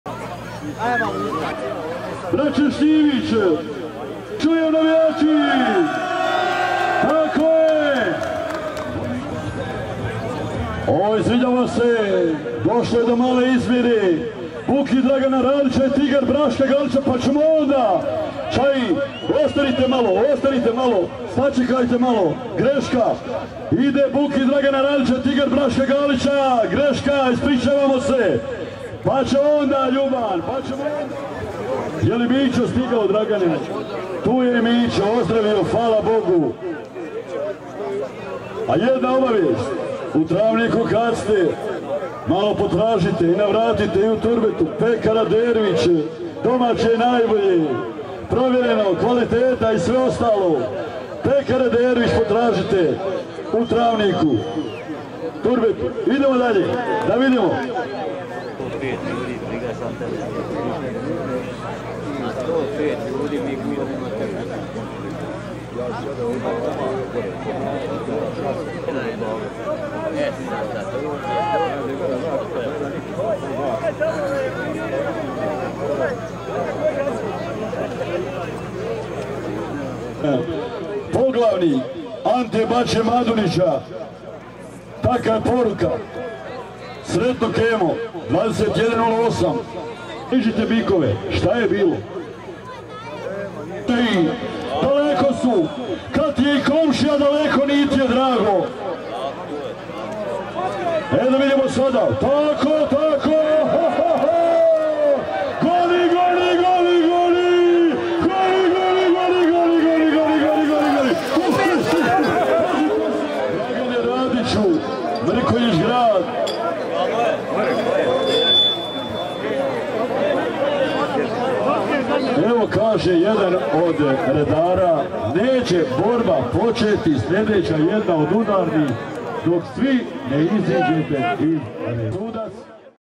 - BUKI OD RADIĆA I TIGAR OD GALIĆA, BUSOVAČA 2022
Kratko ali slatko. Dragan radić sa Bukijem nastupio u Busovači